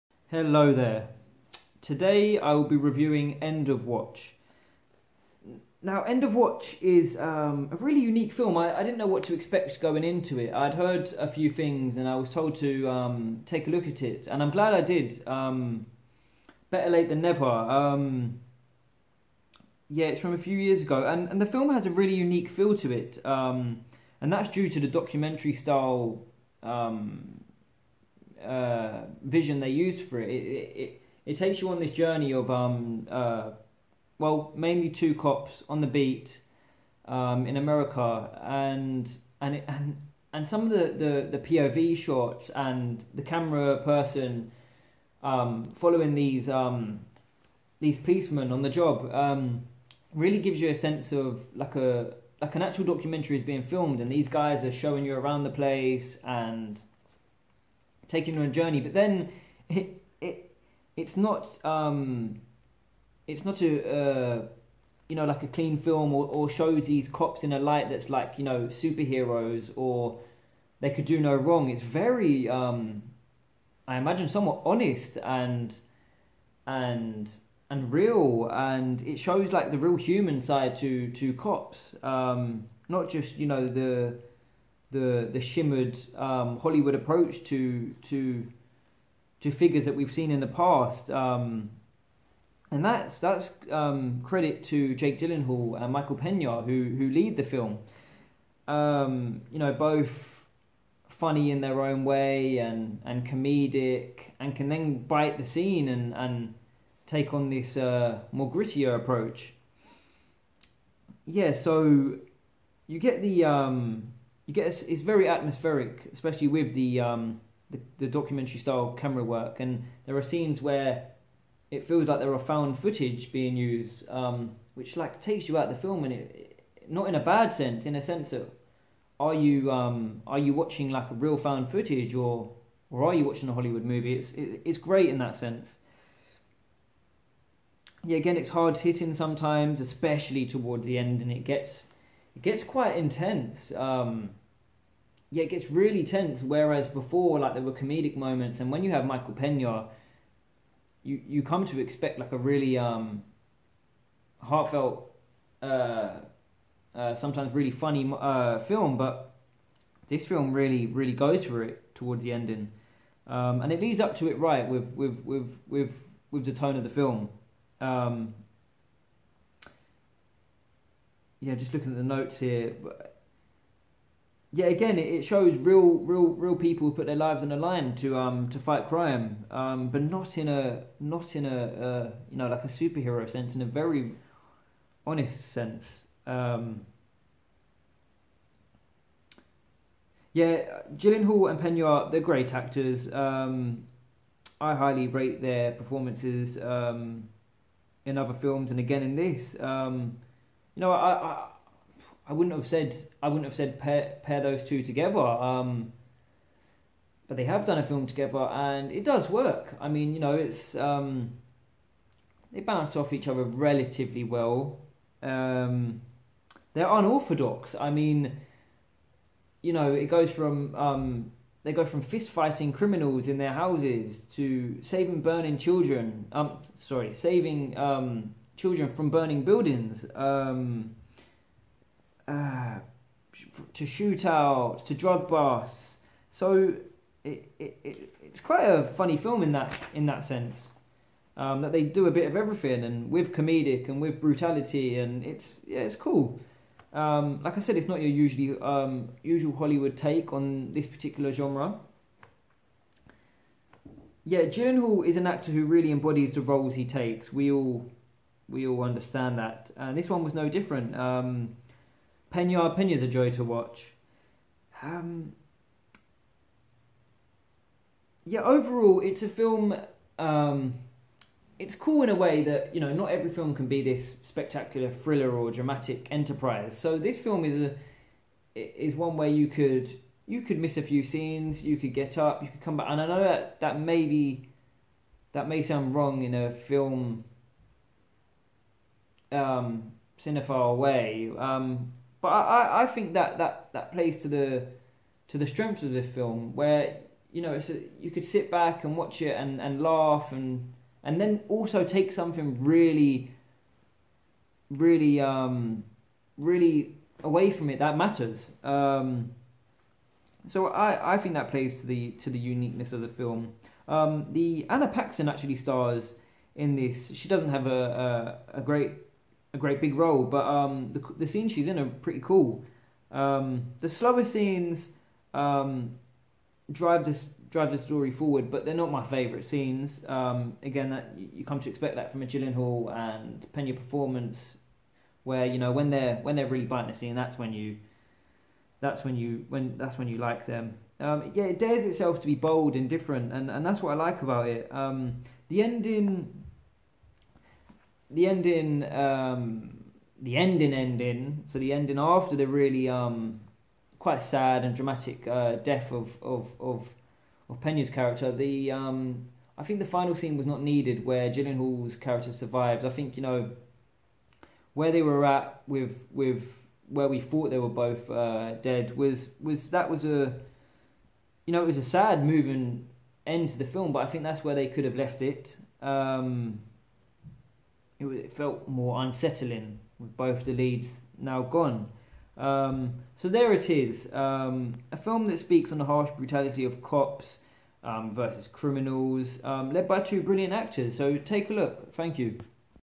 END OF WATCH: MOVIE REVIEW ( spoilers )
hello,

today I will be reviewing:

End Of Watch

starring:

Jake Gyllenhaal, Micheal Pena

All views are soley my own
what's your take ?